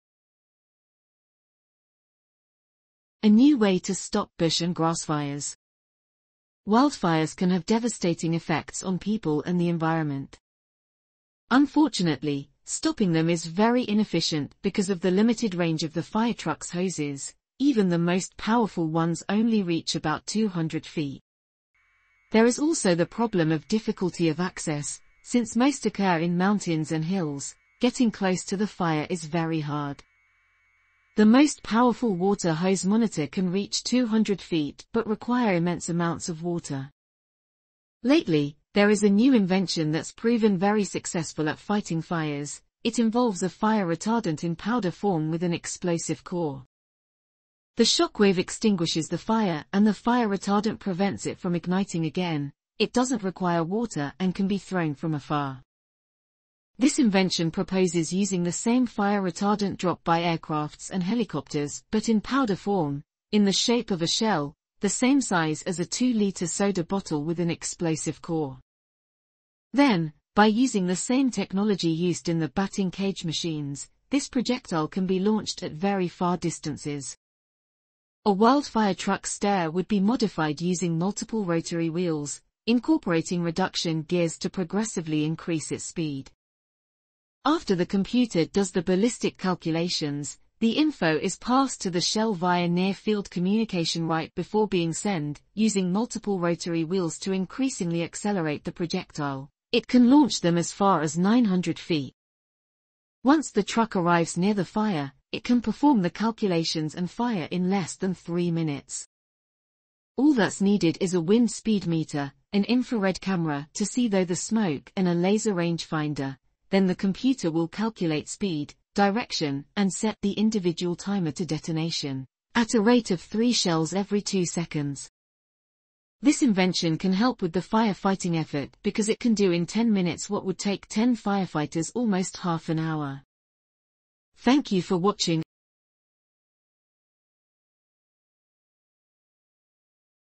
Anti Wildfire truck
This invention relates to a modified Firefighter truck,  in which telescoping ladder, has been repurposed  with multiple spinning wheels to launch a shell consisting of a fire retardant powder with an explosive core
set to detonate right on top of the fire, it  can fire 2 shells every 3 seconds.

This wildfire truck can do in 5 minutes what takes half a dozen brave firefighters almost 30 minutes